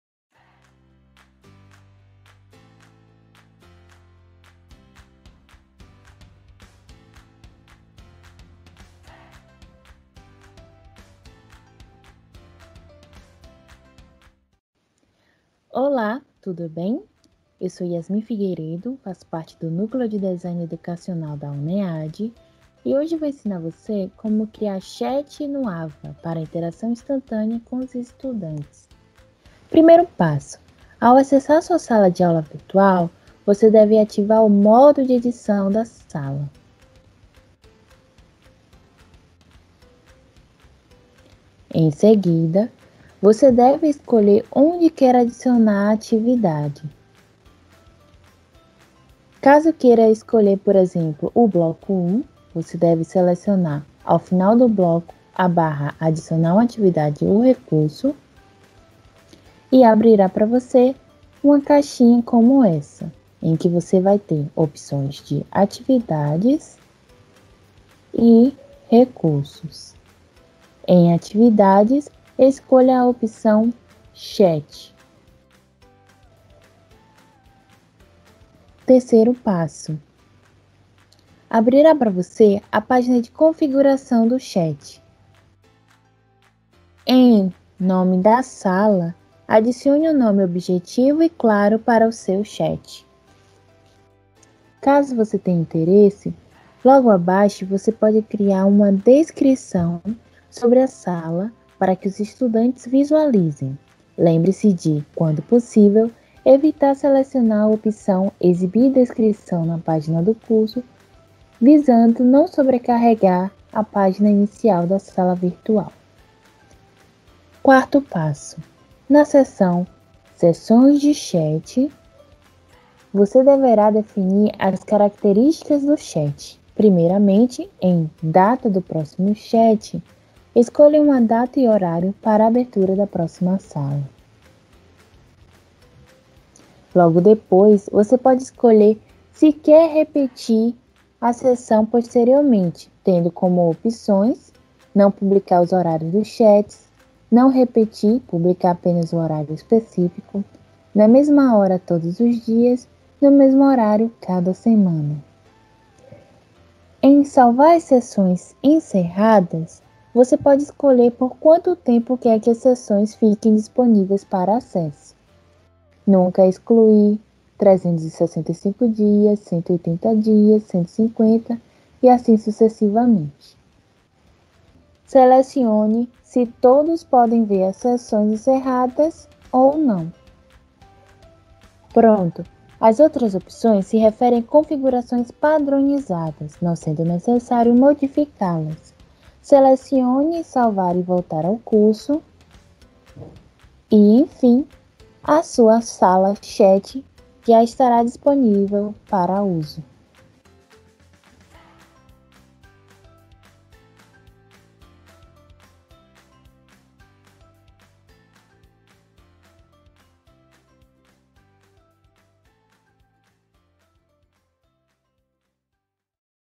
Tutoriais UNEAD: Como criar Chat no Moodle
1º passo: Ao acessar sua sala de aula virtual, você deve ativar o modo de edição da sala. Em seguida, você deve escolher onde quer adicionar a atividade. Por exemplo, caso queira adicionar o questionário no Bloco 1, você deve se direcionar ao final do bloco para a barra denominada “Adicionar uma atividade ou recurso”.

2º passo: Aparecerá para você essa caixinha, contendo opções de “atividades” e de “recursos”. Na parte das “atividades”, selecione a opção “Chat”.

3º passo: Abrirá para você a página de configuração do Chat. Em “Nome da sala”, adicione um nome objetivo e claro para o seu chat. Caso tenha interesse, logo abaixo você pode criar uma descrição sobre a sala para que os estudantes visualizem. Lembre-se de, quando possível, evitar selecionar a opção “Exibir descrição na página do curso” para não sobrecarregar a página inicial da sua sala virtual.

4º passo: Na seção “Sessões de chat”, você deverá definir as características do chat. Primeiramente, em “Data do próximo chat” escolha uma data e horário para abertura da sala. Logo depois, você pode escolher se quer repetir a sessão posteriormente, tendo como opções: “Não publicar os horários dos chats”, “Não repetir – publicar apenas o horário específico”, “Na mesma hora todos os dias” e “No mesmo horário cada semana”. Em “Salvar as sessões encerradas”, você pode escolher por quanto tempo quer que as sessões fiquem disponíveis para acesso. Por fim, selecione se “Todos podem ver as sessões encerradas” ou não.

5º passo: Pronto. As outras opções se referem a configurações padronizadas, não sendo necessário modificá-las. Selecione “Salvar e voltar ao curso” e, enfim, a sua sala “chat” já estará pronta para uso.